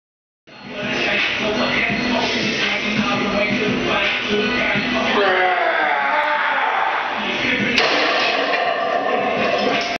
Rack Pulls 575 x 1
It felt great to hold this much weight.  I may have had one more shot left in me, but we'll save it for next workout.